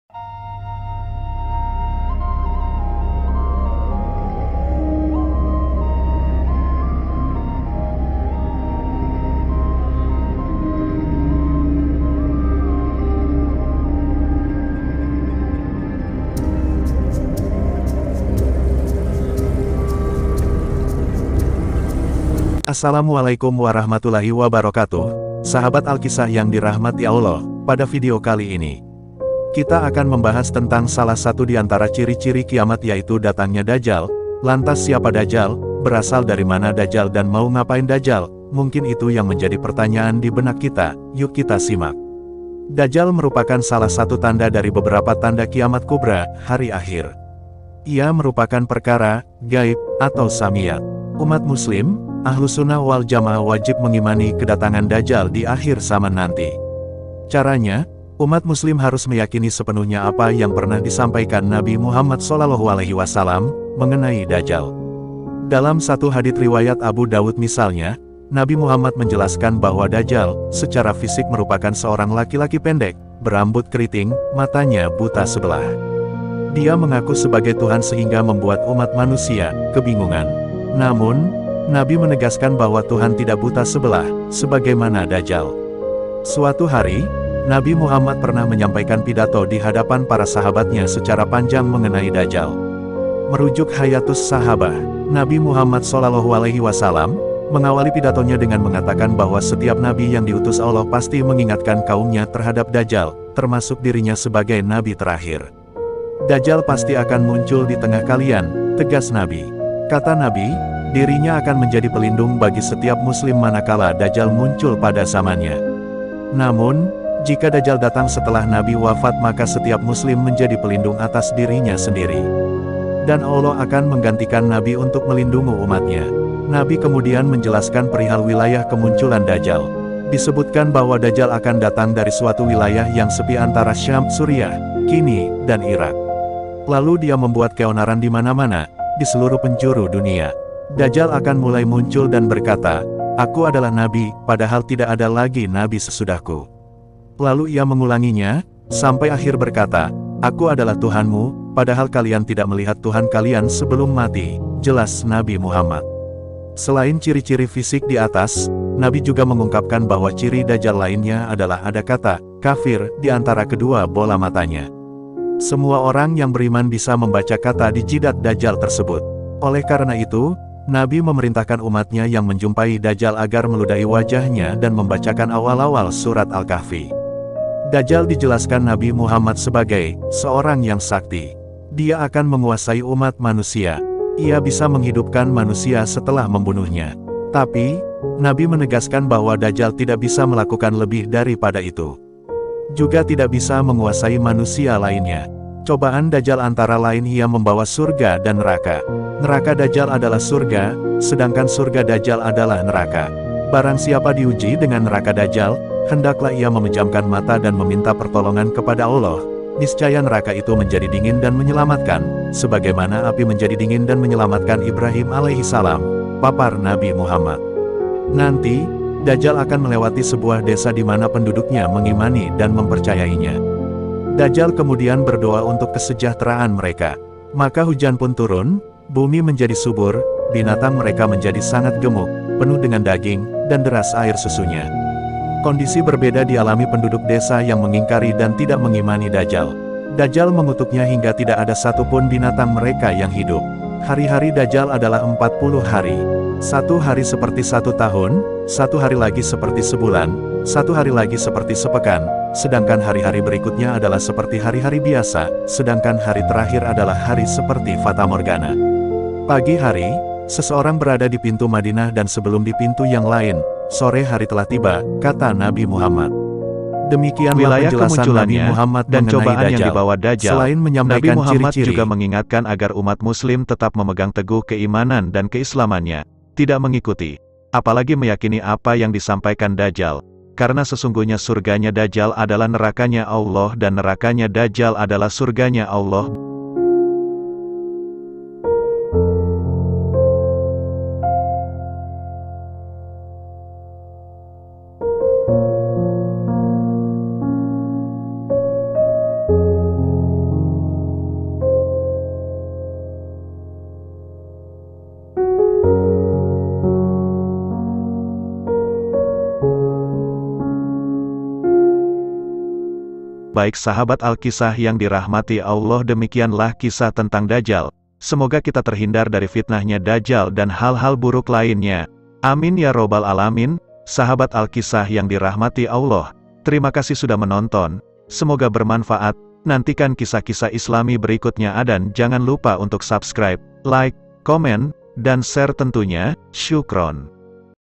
SIAPAKAH DAJJAL..???
siapakah Dajjal,?dari mana Dajjal?dan mau ngapain Dajjal